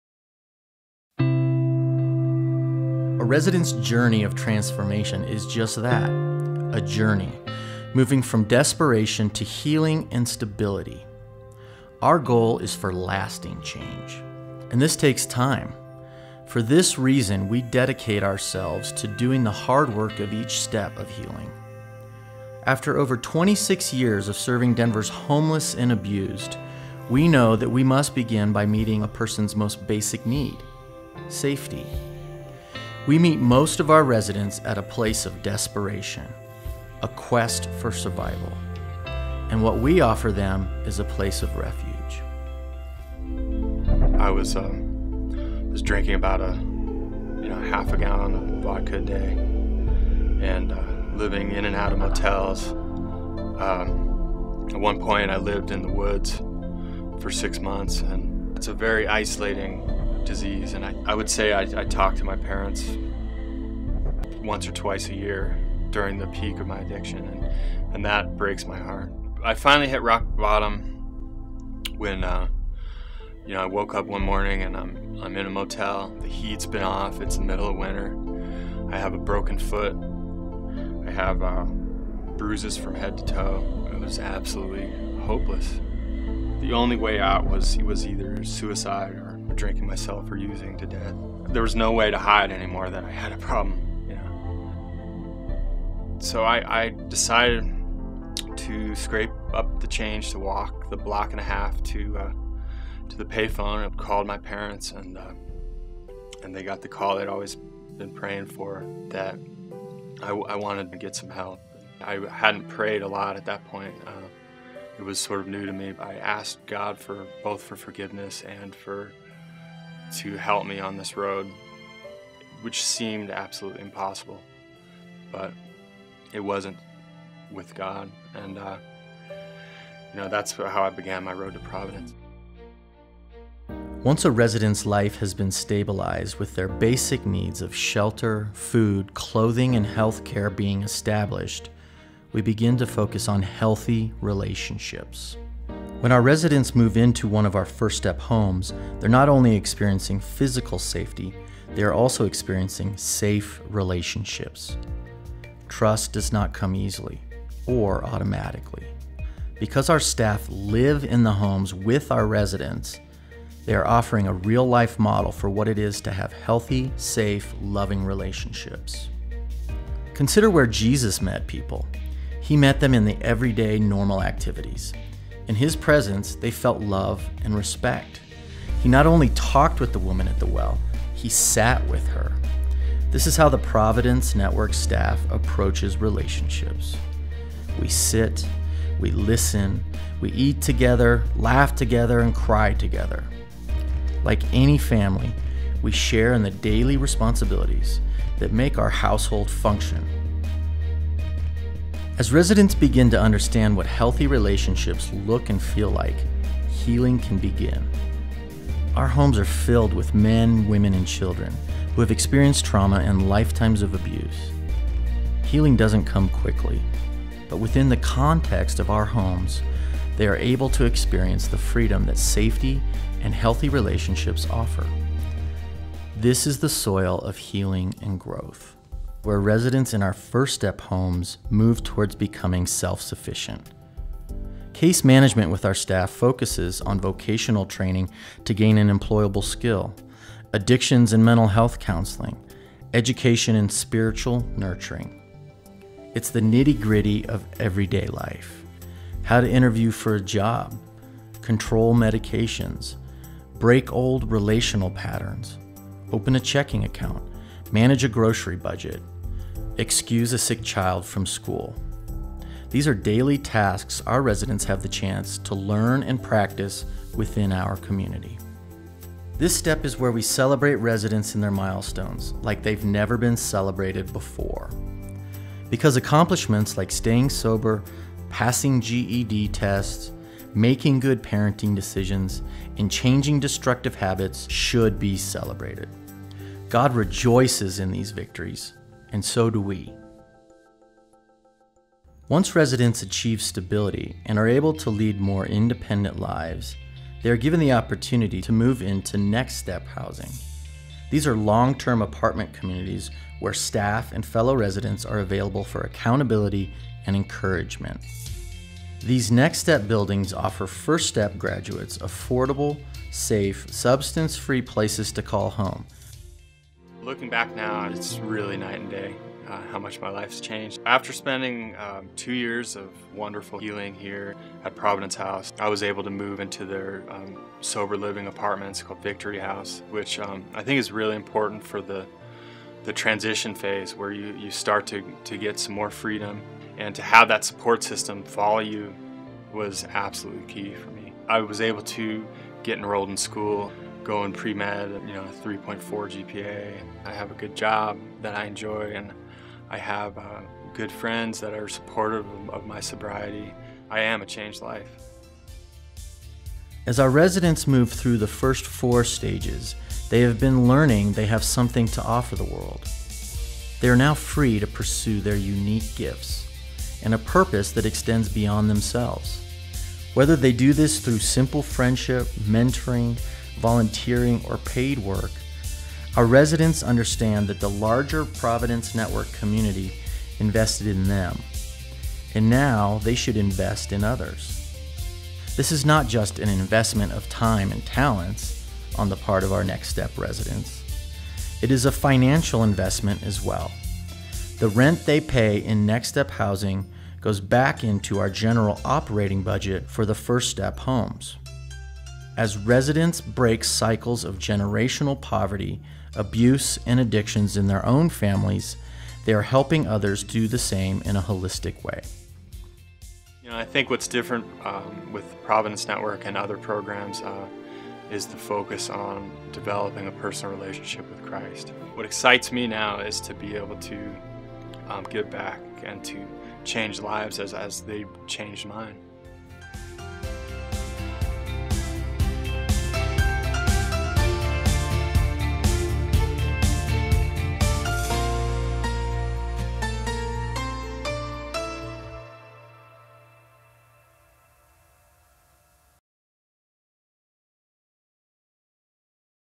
A Resident's Journey of Transformation - Spencer's Story | Denver, CO nonprofit
Providence Network is a Denver faith-based, urban transformational housing organization that helps men, women and families heal from addictions and abuse.